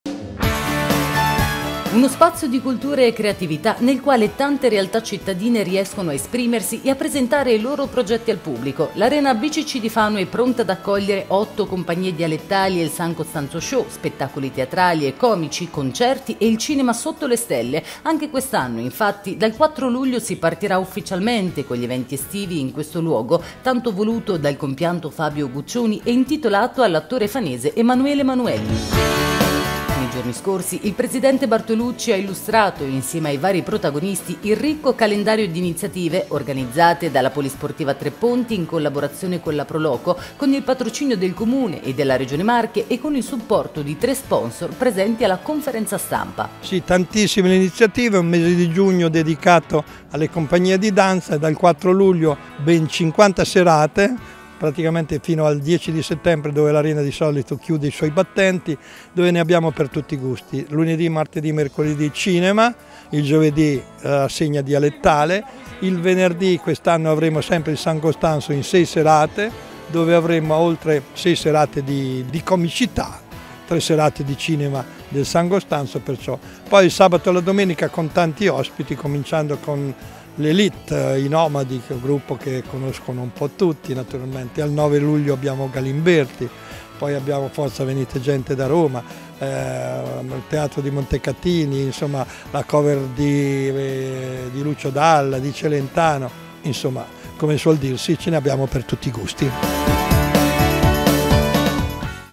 Arena BCC Fano: presentati gli eventi 2024. 50 eventi in due mesi
50 eventi in due mesi, tra concerti, spettacoli teatrali e cinema: il 4 luglio al via il programma degli eventi estivi dell'Arena BCC a Tre Ponti di Fano.